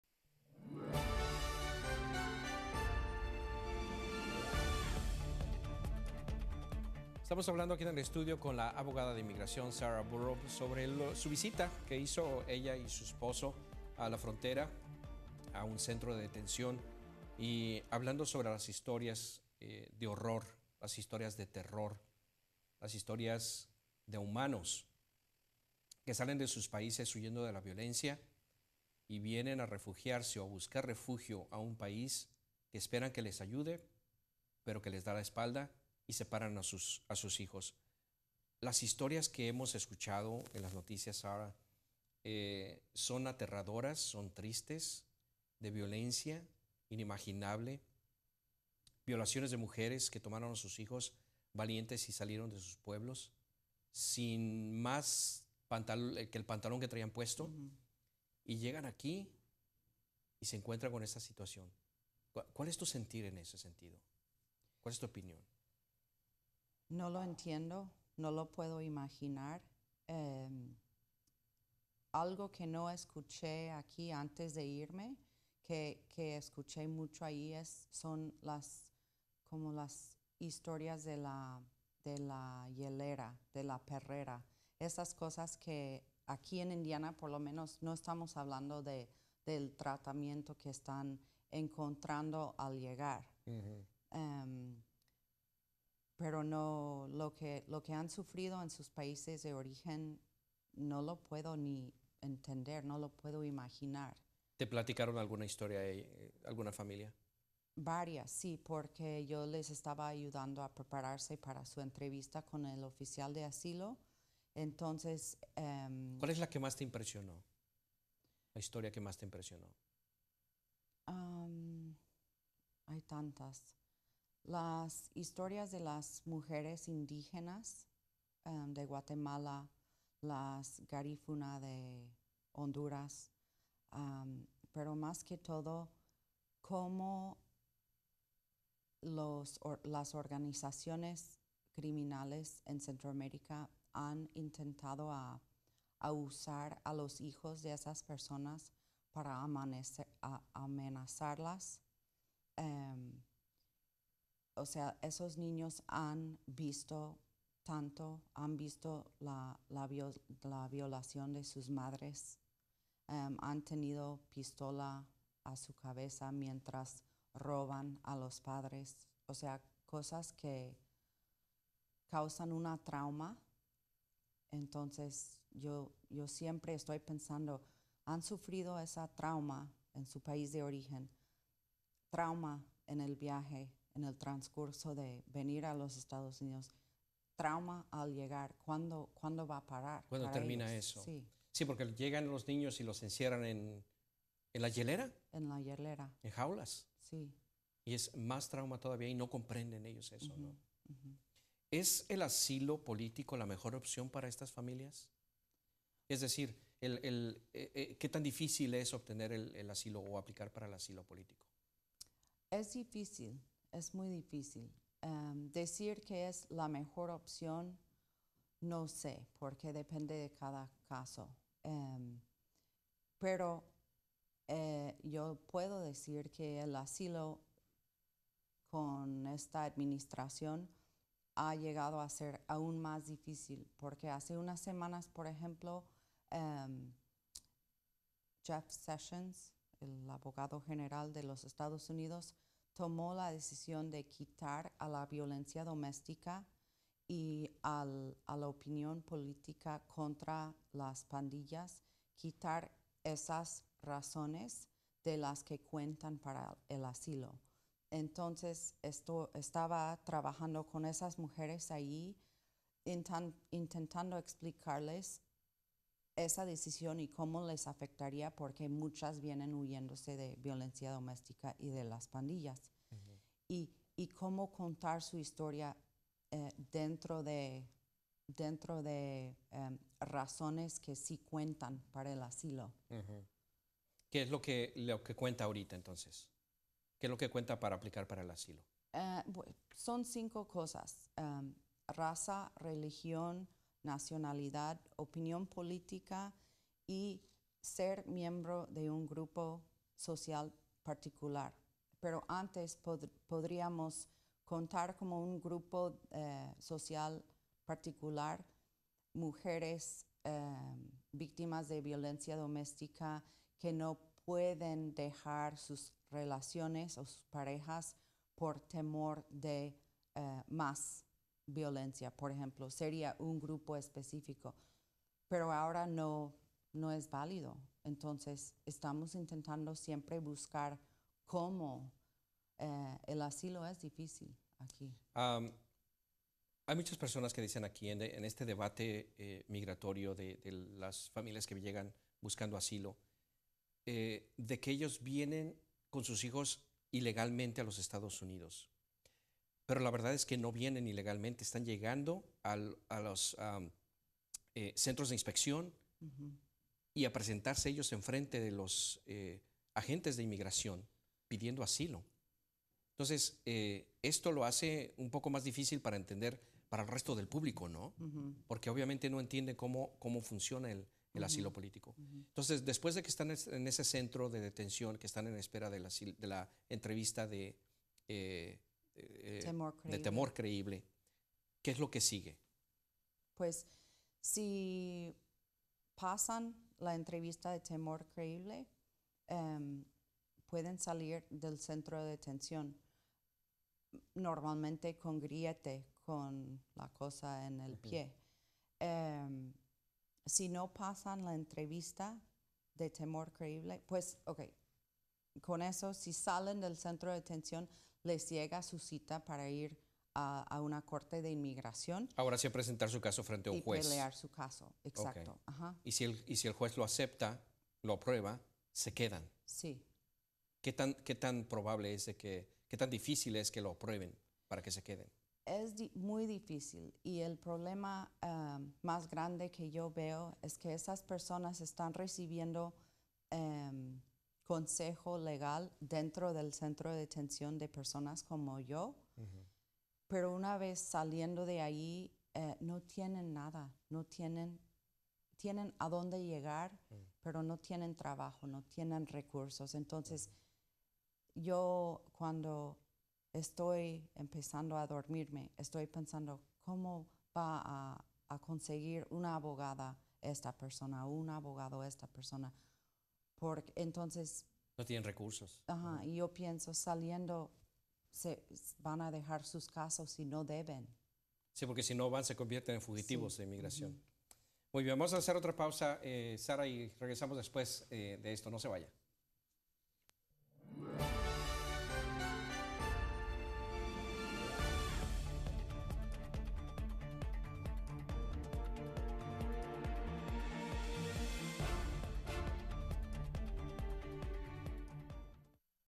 Abogada Sarah Burrow comparte experiencia de visita a centro de detención. PARTE 3
La abogada de inmigración de Indianápolis, Sarah Burrow habla con Luis Navarro sobre su experiencia durante su visita a un centro de detención migratorio en la frontera con México. PARTE 3.